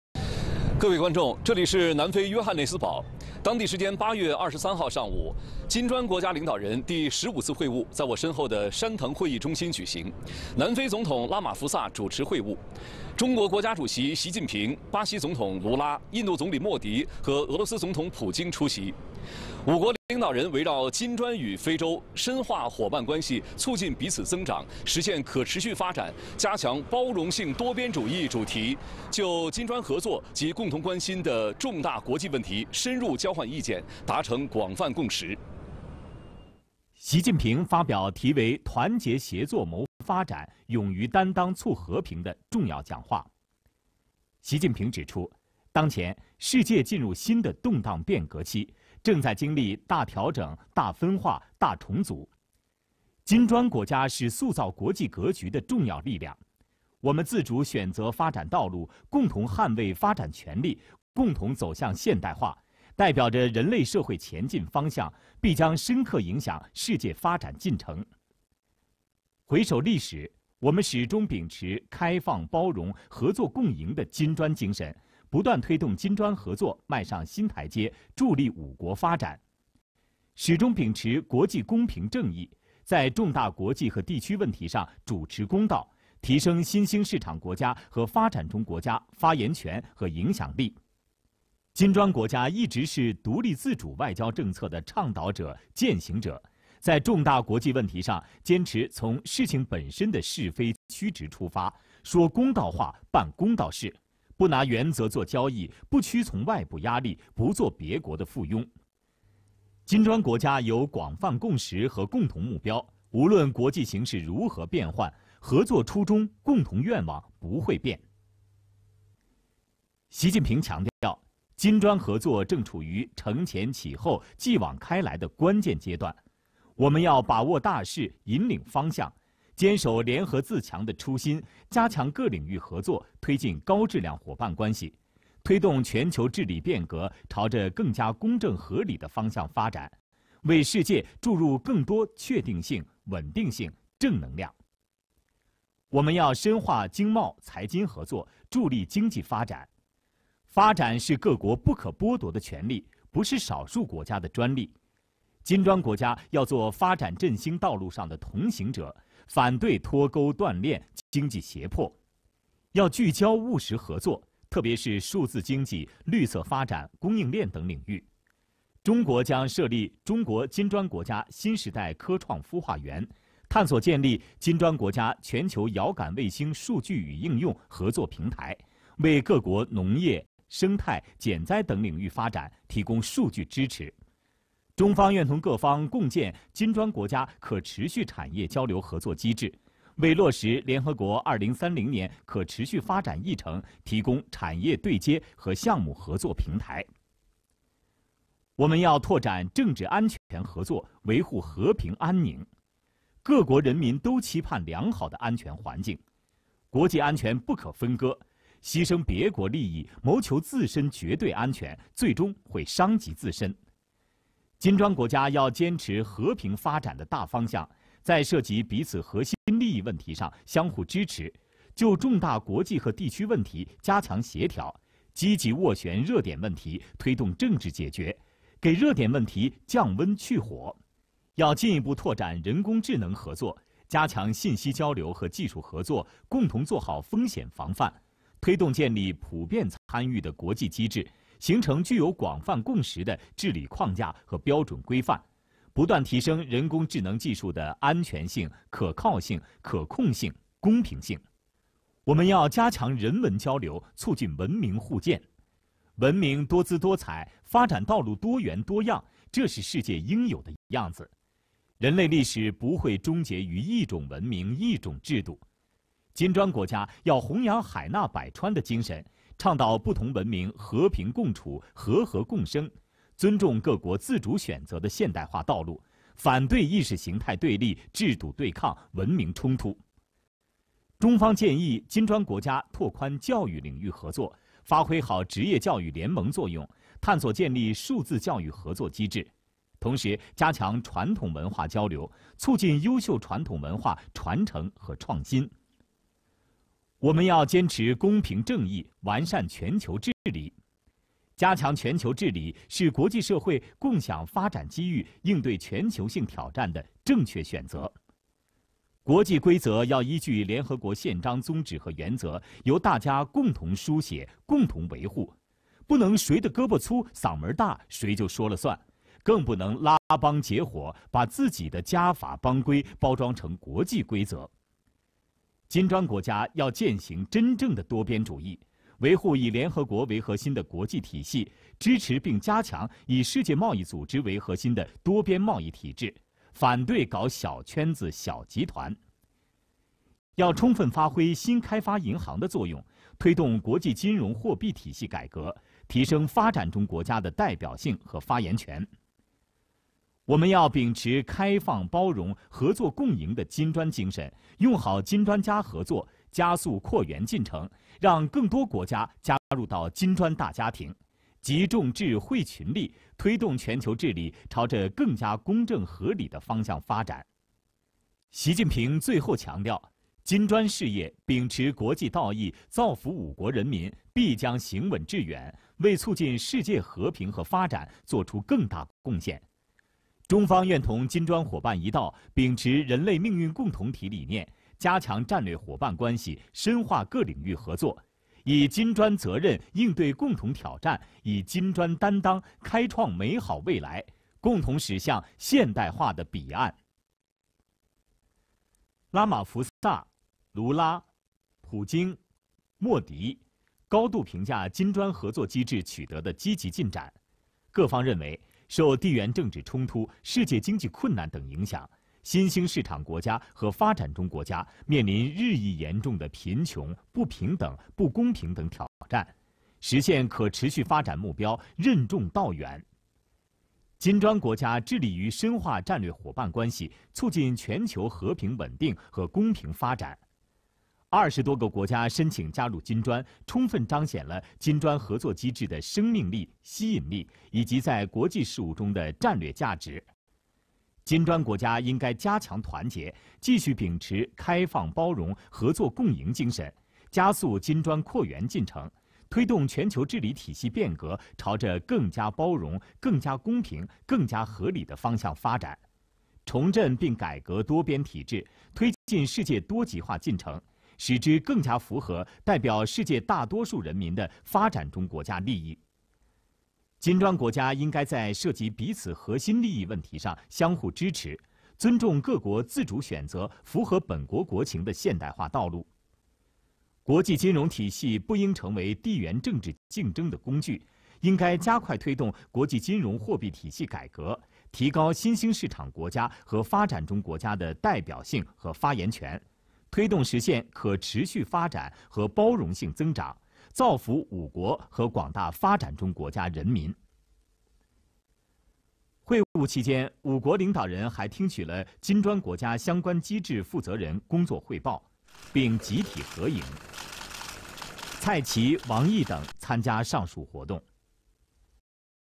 习近平出席金砖国家领导人第十五次会晤并发表重要讲话
当地时间8月23日上午，金砖国家领导人第十五次会晤在约翰内斯堡杉藤会议中心举行。南非总统拉马福萨主持会晤。中国国家主席习近平、巴西总统卢拉、印度总理莫迪和俄罗斯总统普京（线上）出席。五国领导人围绕“金砖与非洲：深化伙伴关系，促进彼此增长，实现可持续发展，加强包容性多边主义”主题，就金砖合作及共同关心的重大国际问题深入交换意见，达成广泛共识。

习近平发表题为《团结协作谋发展 勇于担当促和平》的重要讲话。（视频来源：中央广播电视总台）